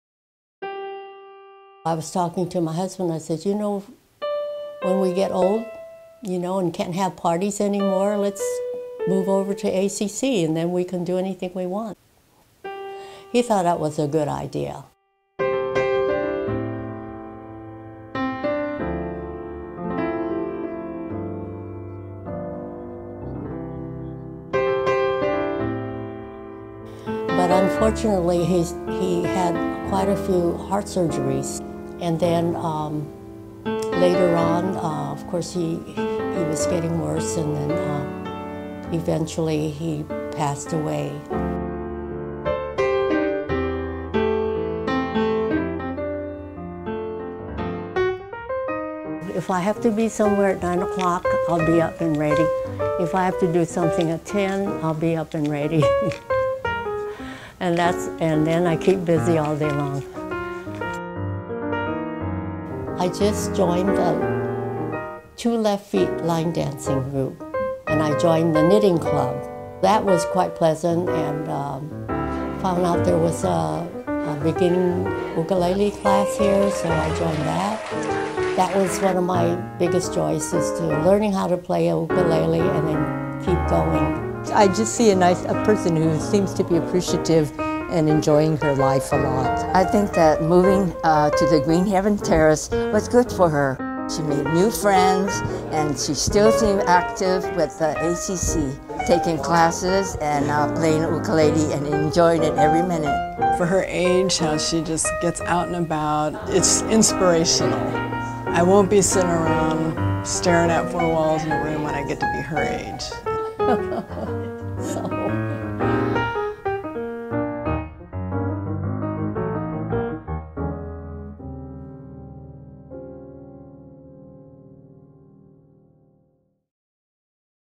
There's no speed limit at 86
She drives her own car, plays ukulele, sings, does line dancing and more. At 86, Elsie Yun is putting the pedal to the medal. Presented by ACC Senior Services.